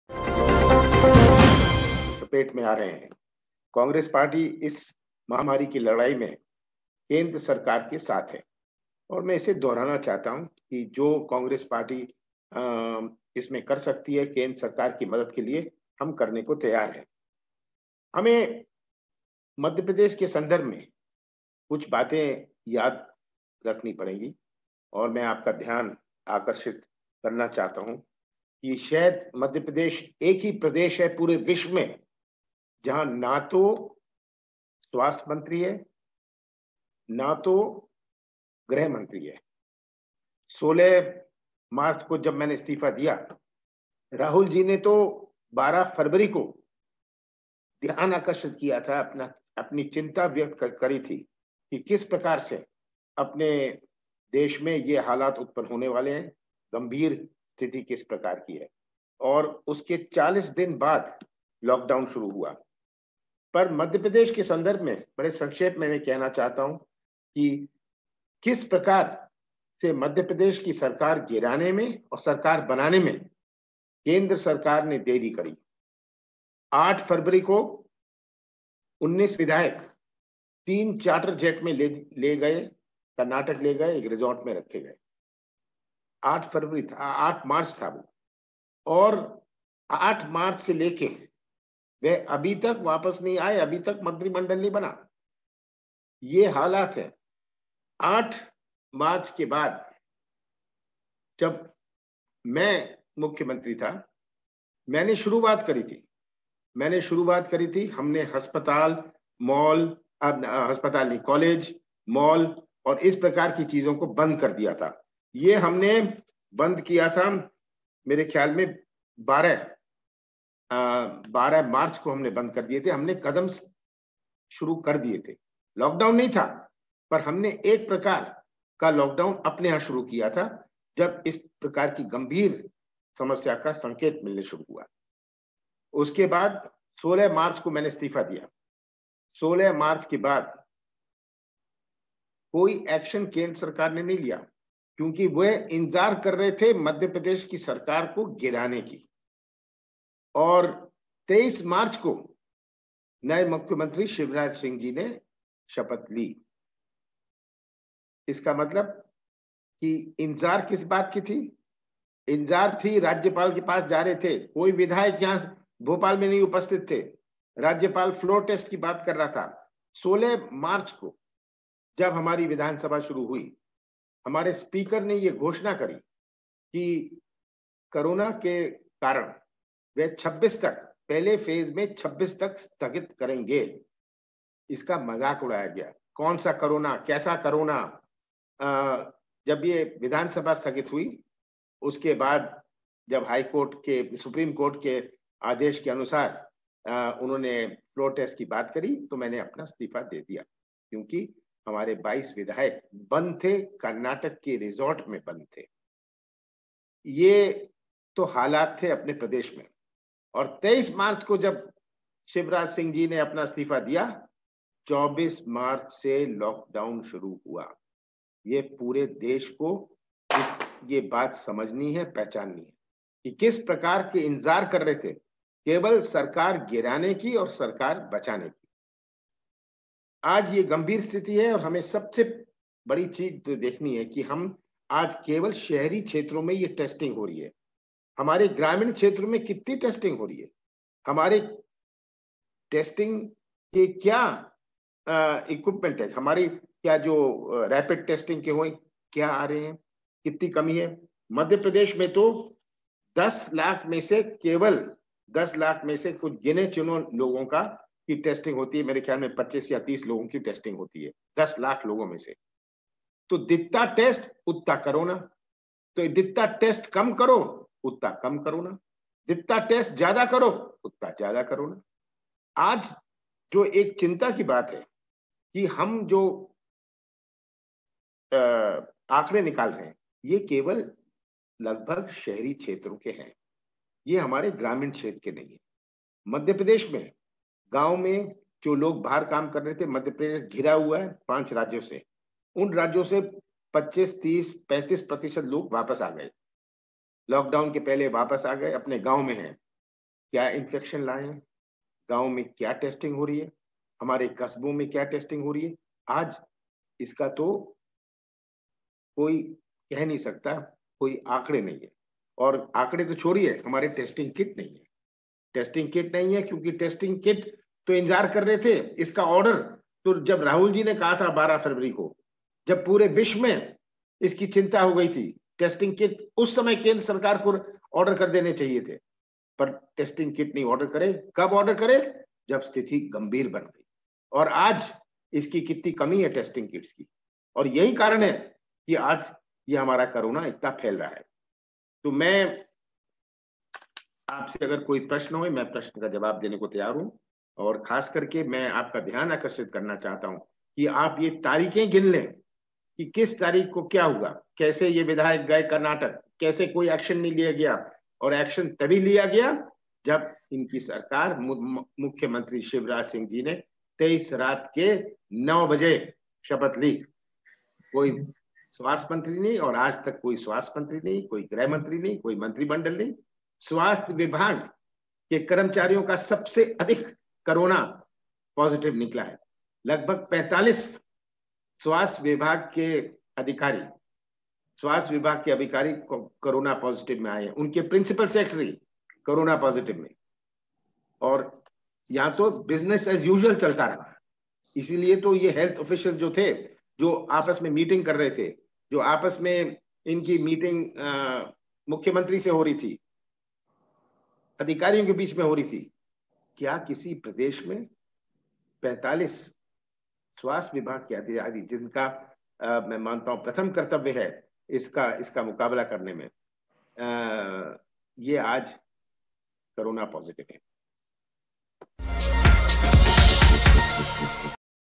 Lockdown delayed to topple Madhya Pradesh government: Kamal Nath
Former Madhya Pradesh CM and senior Congress leader Kamal Nath on Sunday alleged that Bharatiya Janata Party-led central government delayed imposition of lockdown over Covid-19 to ensure their government is formed in the state. Nath was addressing a press conference through video-conferencing.